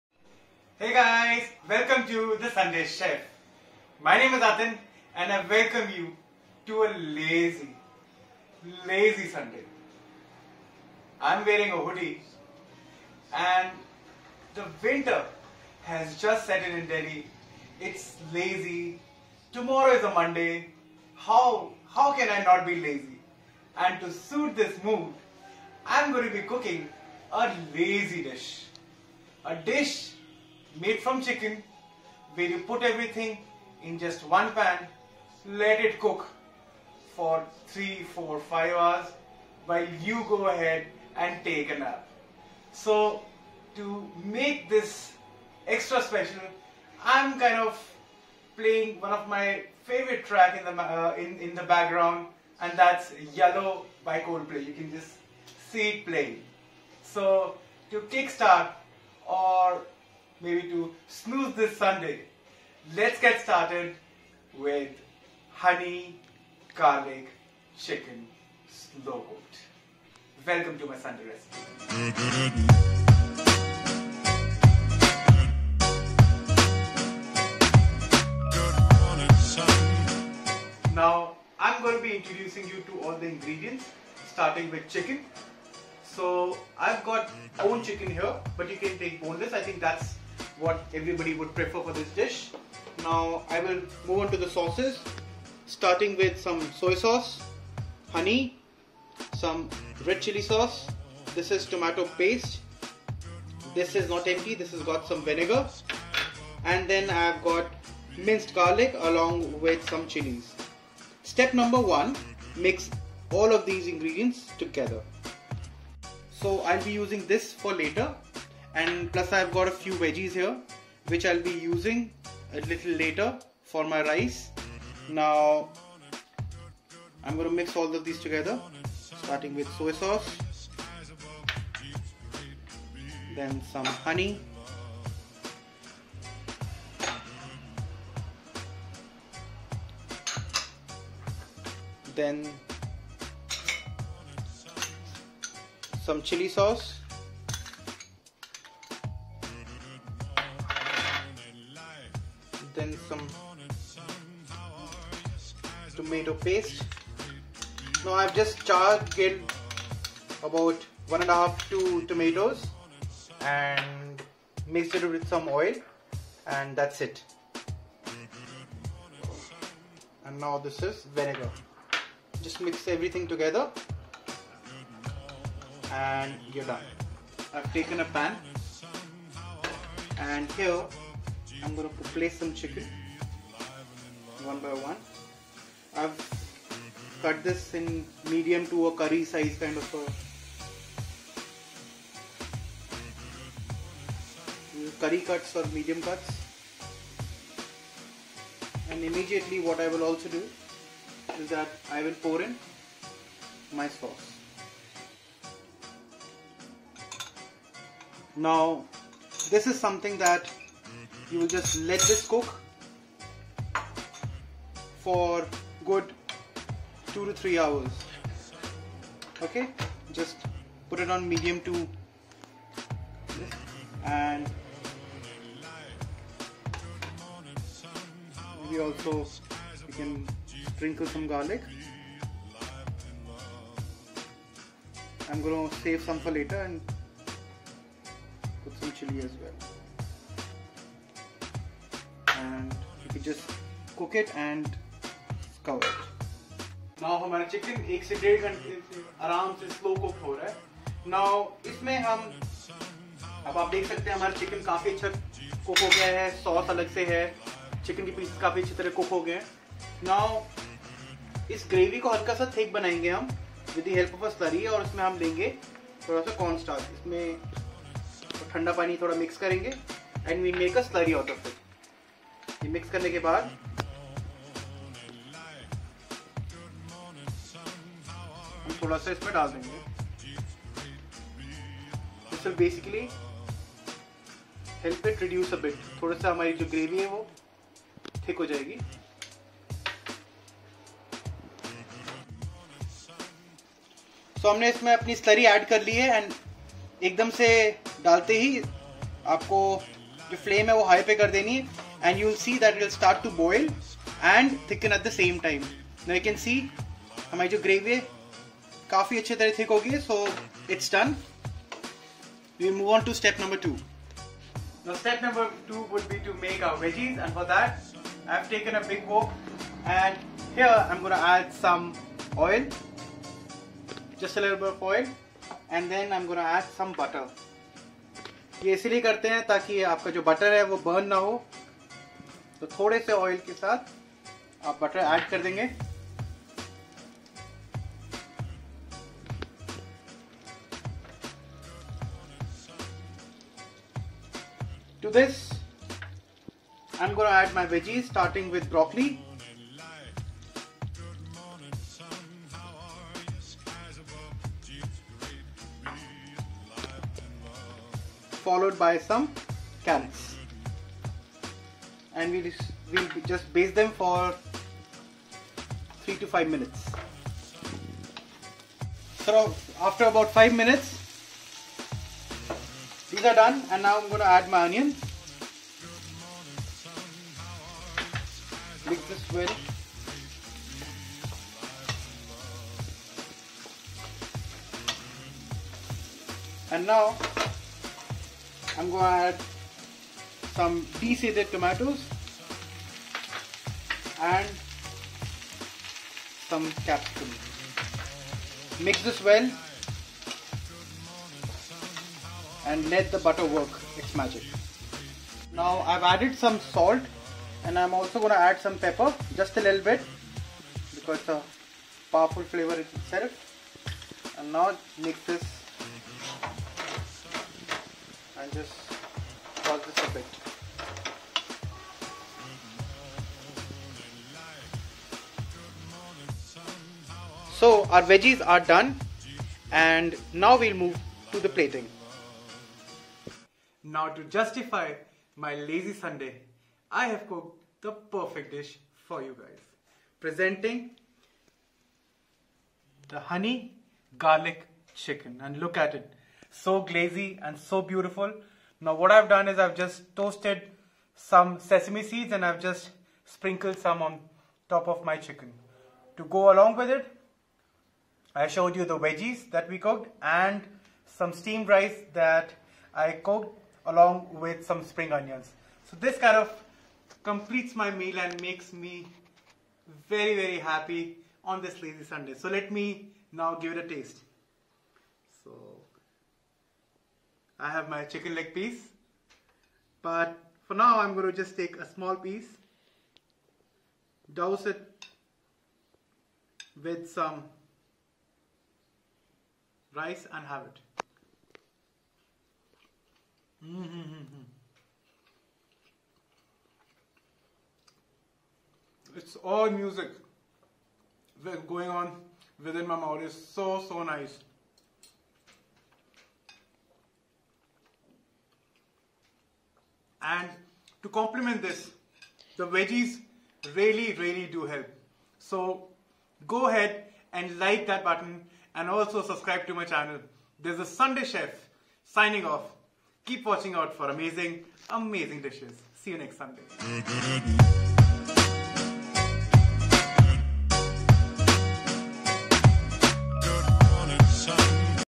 TSC - Slow Cooked Honey Garlic Chicken - Lazy Sunday Special -
Do you feel lazy too?

Presenting a Super Lazy, Super Easy - Honey Garlic Chicken.

I am sure you will love it.

Ingredients:-
For the Chicken:
500gms  boneless/ Bonechicken, skinless chicken thighs or chicken breasts
1/4 cup soy sauce
1/4 cup honey
2 tablespoons tomato paste
2 teaspoons chili paste
8-10 cloves garlic minced
2 slot green chilly
1 tablespoon  vinegar
2 tablespoons cornstarch

Recipe:-
Instructions

1. Place the chicken in the bottom of a cooker/ pan. In a medium mixing bowl or very large measuring cup, whisk together the soy sauce, honey, tomato paste, chili paste, garlic, and rice vinegar. Pour over the chicken. Cover and cook on LOW for 4 to 5 hours or HIGH for 1-2 hours, until the chicken get tender. If you are available, flip the chicken over once halfway through to coat both sides. (If not, don’t stress; it will still be tasty.)

2. Whisk the cornstarch into the slow cooker cooking liquid. Cover and cook on HIGH for 15 minutes, until the sauce thickens slightly, stirring occasionally. If you'd like the sauce particularly thick, you can cook it for a full 30 minutes in the slow cooker OR follow the stovetop method below.
3. For quicker sauce thickening, reduce the sauce on the stove: After whisking in the cornstarch, transfer the cooking liquid to a medium saucepan. Cook on the stovetop over medium heat, stirring often until the sauce thickens, 5 to 10 minutes. (If your slow cooker insert is stovetop safe, you can remove it from the slow cooker and place it directly on the burner, but do not do this unless you are POSITIVE your insert is stovetop safe or it may crack.)
4. With two forks (or your fingers if the chicken is cool enough), shred the chicken and place it in the slow cooker. If you reduced the sauce on the stove, add it back to the slow cooker now. Stir to coat the chicken with the sauce. Serve over rice, sprinkled with green onions and sesame seeds.